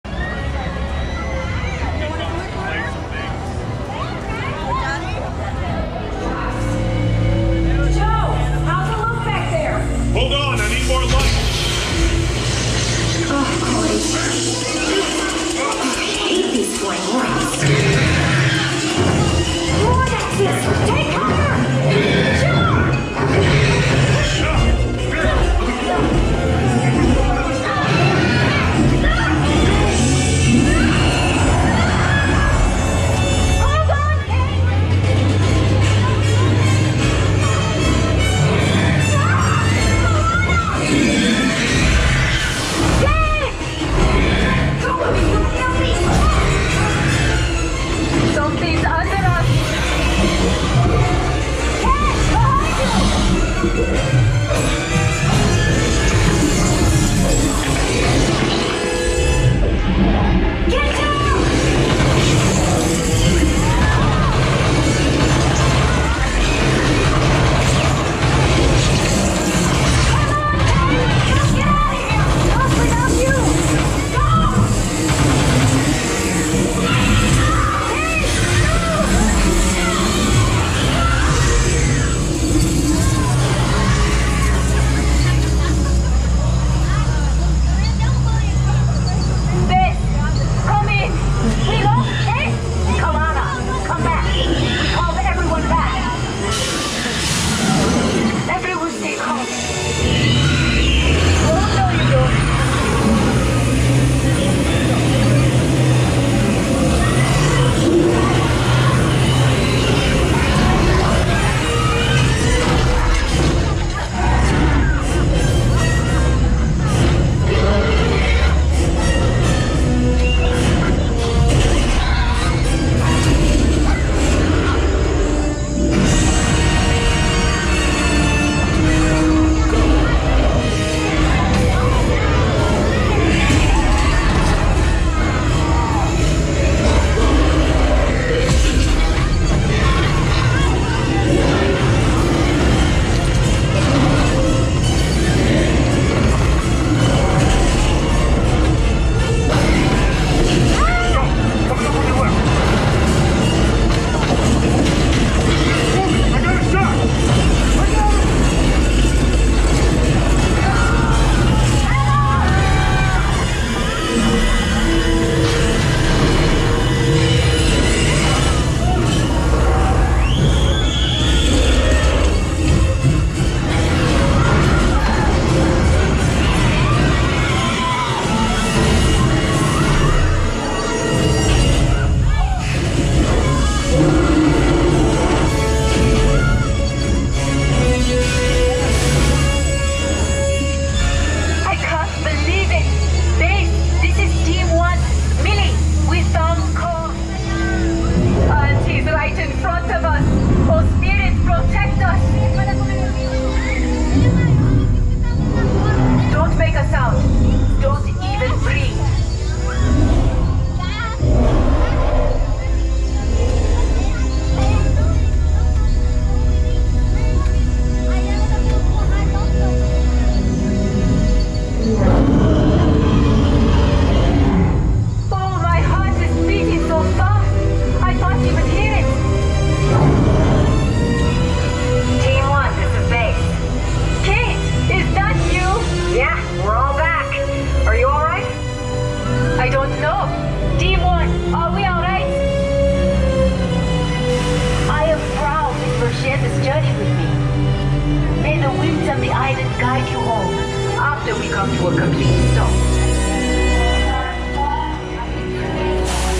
Skull Island: Reign of Kong | Islands of Adventure [2023]
Check out the full ride of Skull Island in Islands of Adventure theme park.